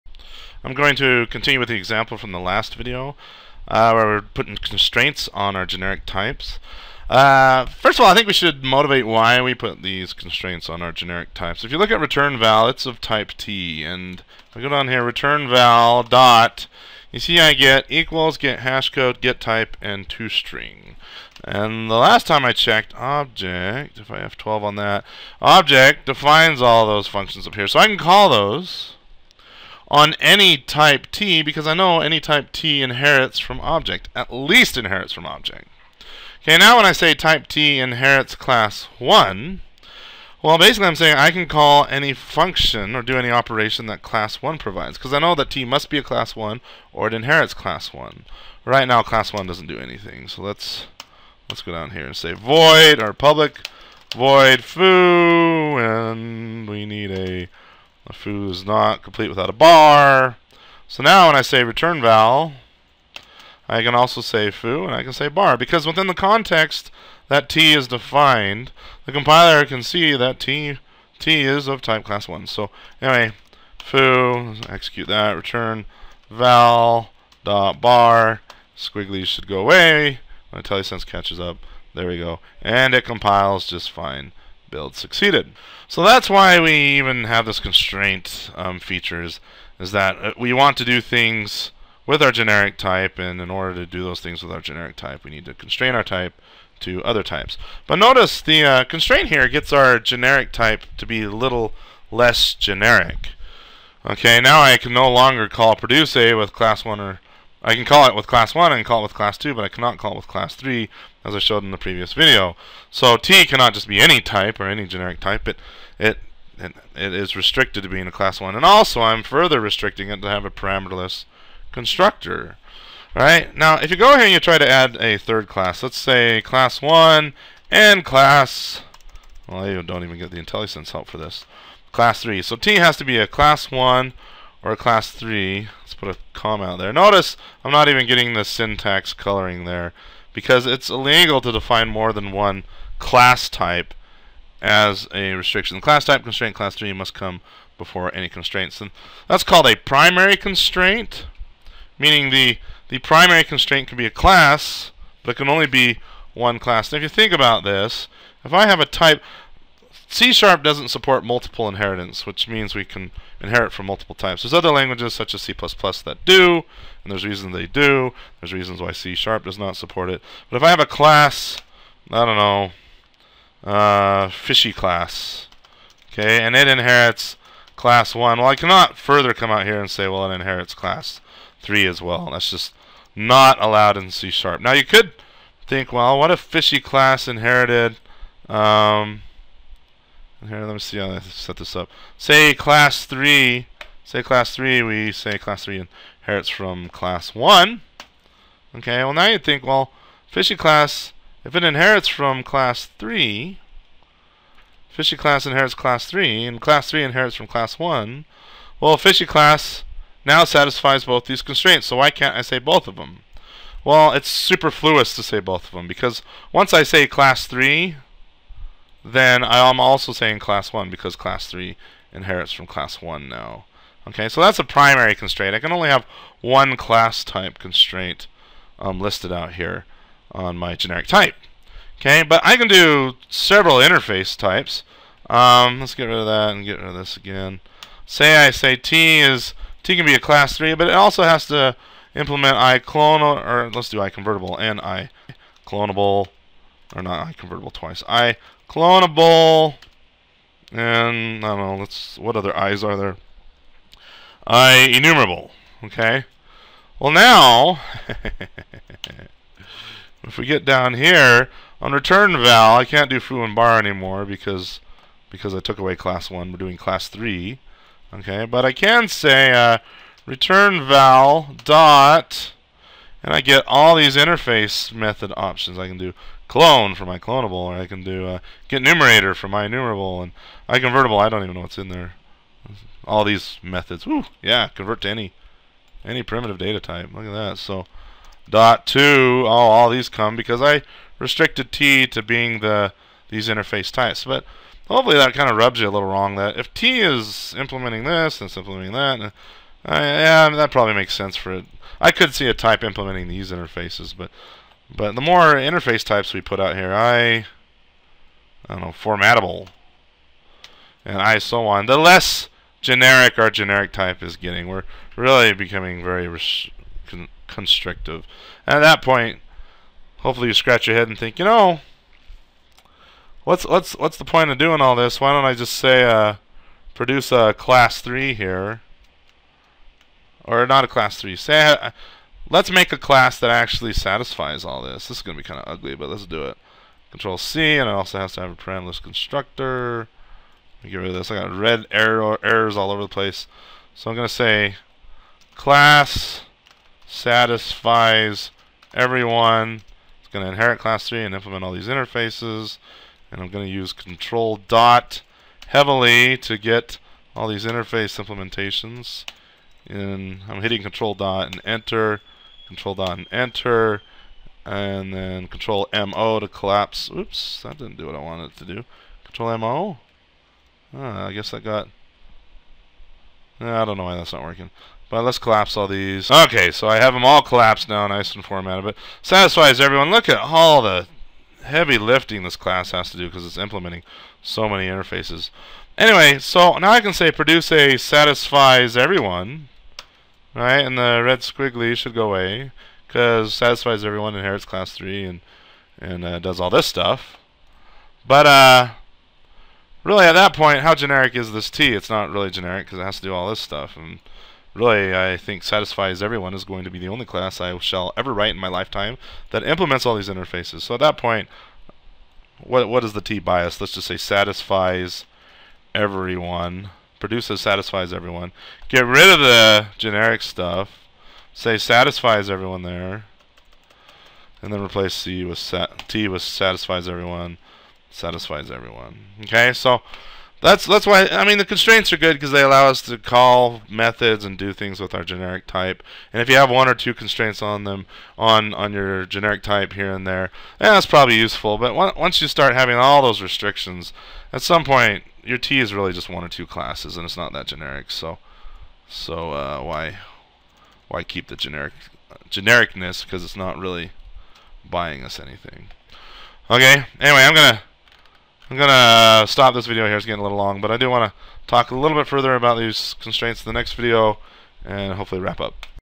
C# Generic Constraints Part 2
Jamie King enumerating the various possible generic constraints.